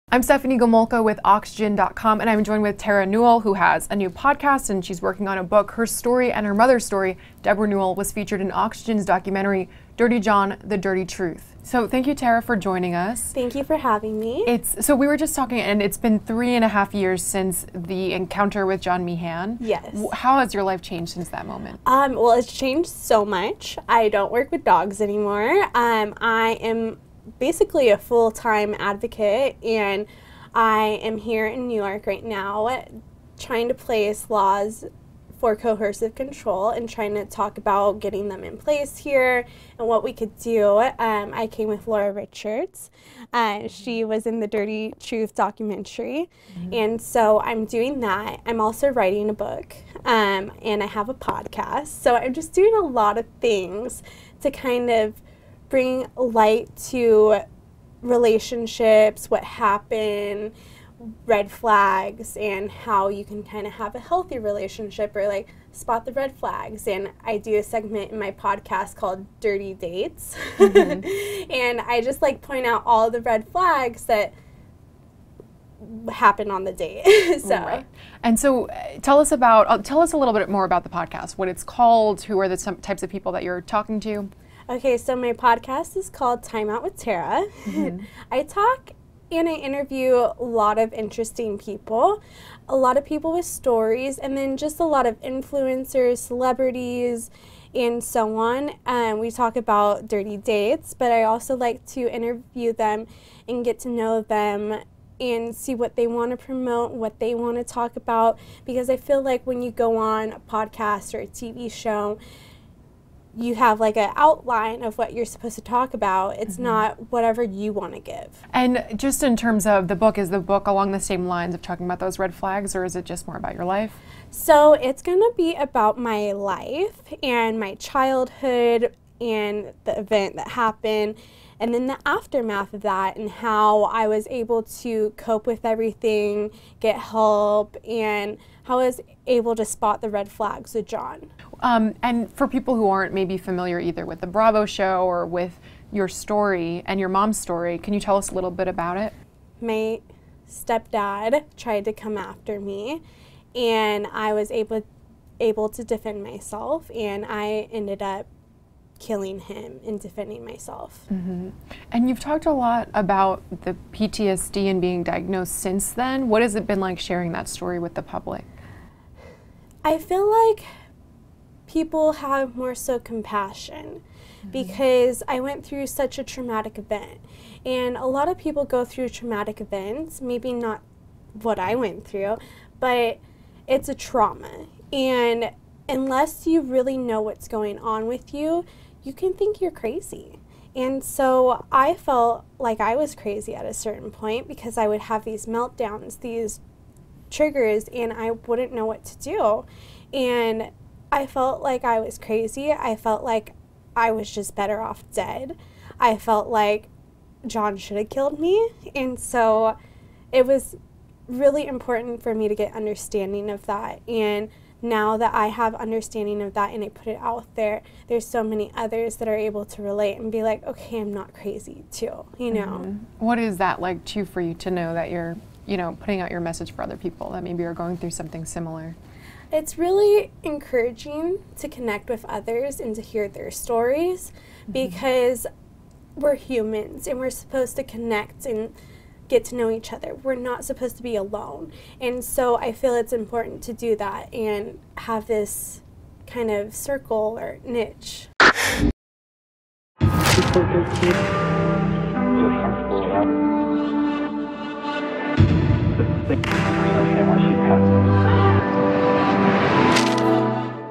Terra Newell Survived A Brutal Attack by Her Stepfather "Dirty John" | Oxygen
Terra Newell survived her estranged stepfather John Meehan attacking her and killed him in self-defense. Her story as well as her mother's, Debra Newell, inspired the show "Dirty John." Newell discusses her upcoming book, new podcast, and her advocacy work.